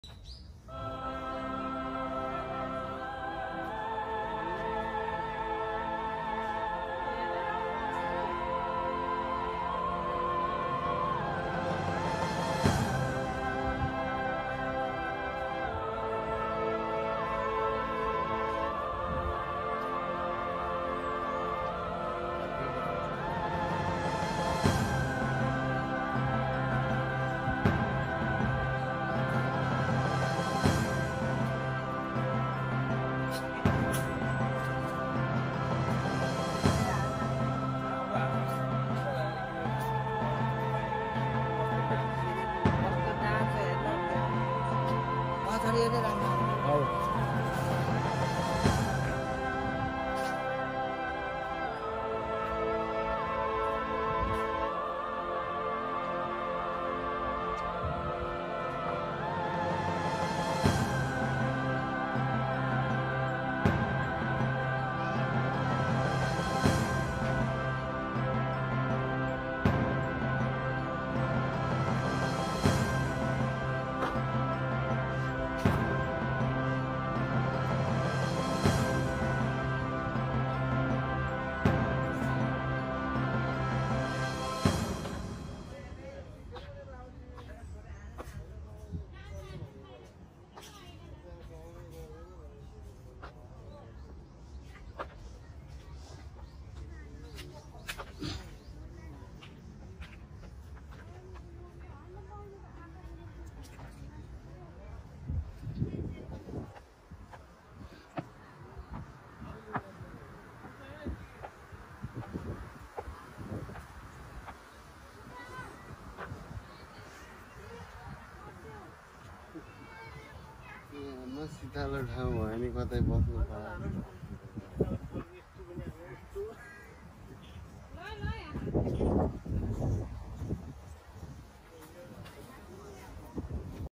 Onoyama park with clz friends vlog in okinawa japan
Very happeness place in okinawa japan. The tropical island of okinawa,
The southernmoct,prefecture of japan are a popular tourist destinations

The video is from Onoyama park place,the main traveling Naha-Shi popular tourist destinations

   I hope you liked this video from okinawa japan

Channel. If you wantto see more travelling video